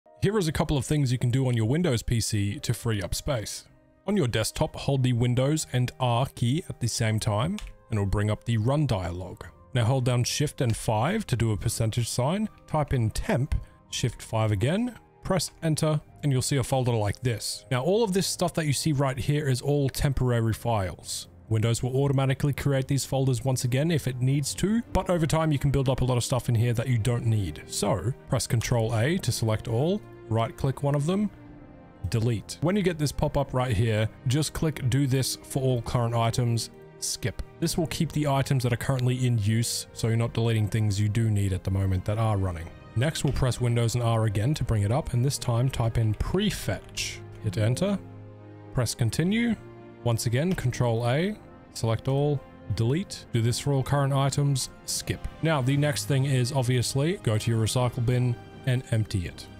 How To Quickly Free Up Space On Your PC With Run Command.
Here's a quick and easy way to free up space on your PC with Windows Run Command. This will allow you to delete a build-up of temporary files. Windows automatically re-creates these folder when needed, but over time you can end up with a TON of stuff you don't need anymore.